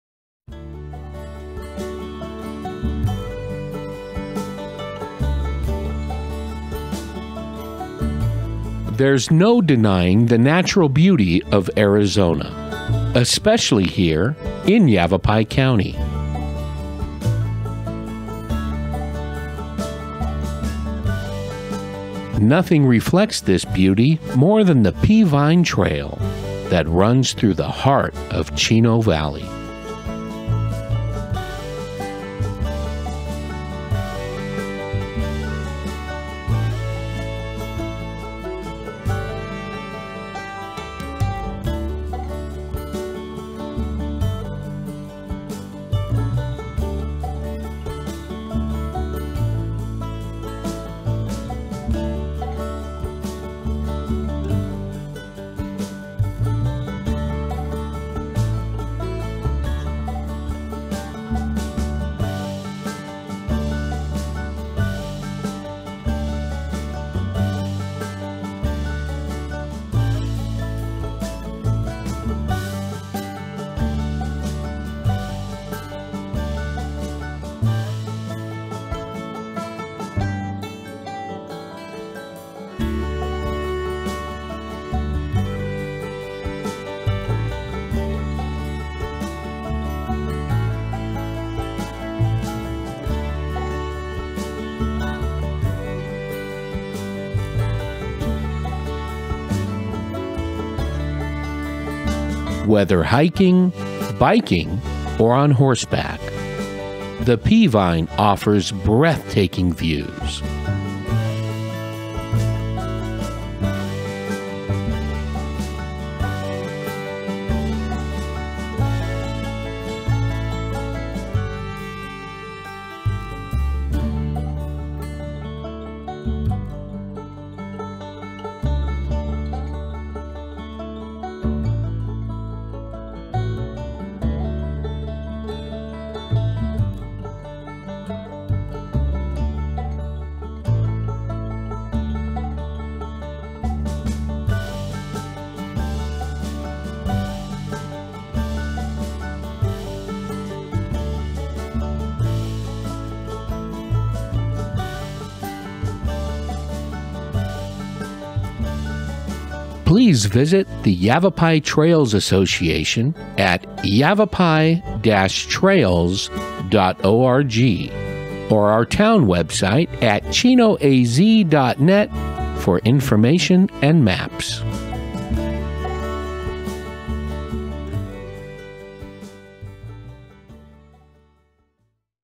The Peavine Trail
The Peavine Trail is a true gem of Yavapai County. Thank you to the Yavapai Trails Association for all of the hard work in keeping this beauty available to all.
Visit yavapai-trails.org or chinoaz.net for info & maps.